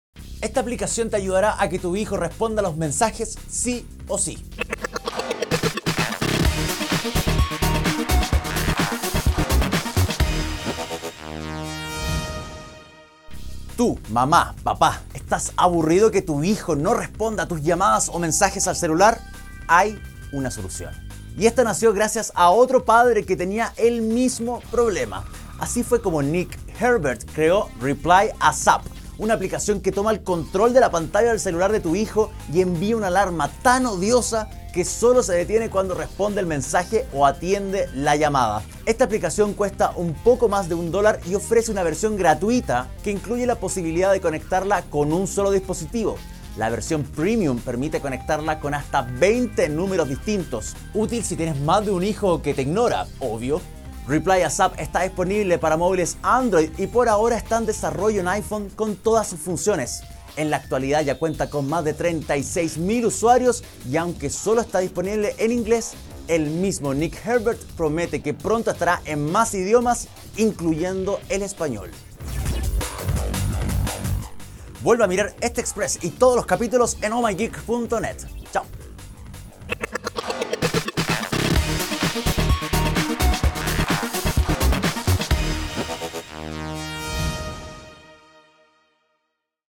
ReplyASAP, una App que obligará a otros a responder tus llamadas y mensajes
Nick Herbert es un papá que estaba aburrido de que su hijo no respondiera sus llamadas. Por lo mismo creó una aplicación que toma el control del teléfono y lo suelta cuando responde.